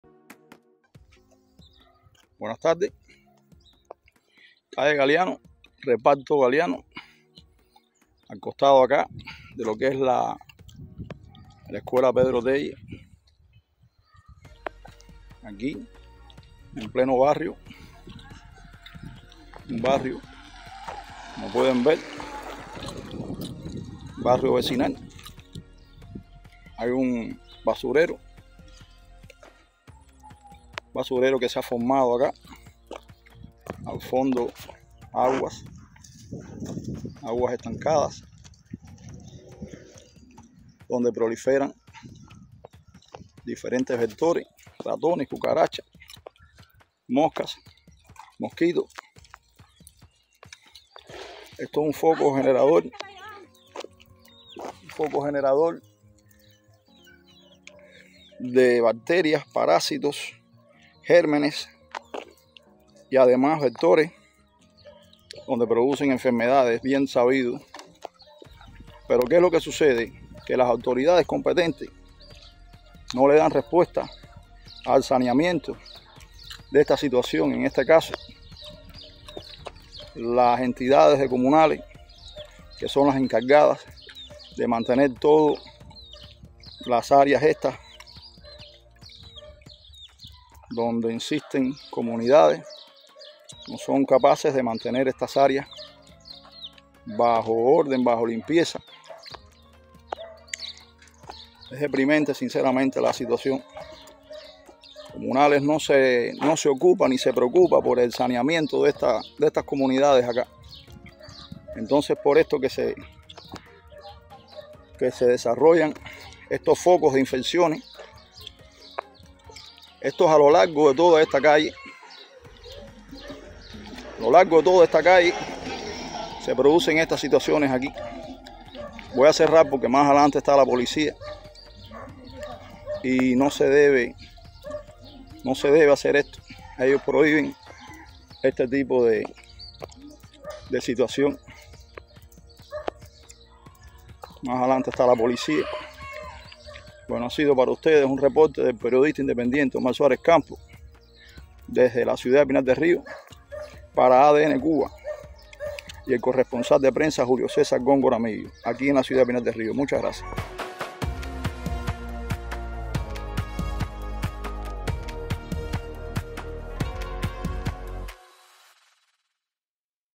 Foco de enfermedades en barrio residencial
Foco de enfermedades en el reparto Galeano de Pinar del Río. Las autoridades no se ocupan de estas comunidades,  según la denuncia enviada a ADN Cuba.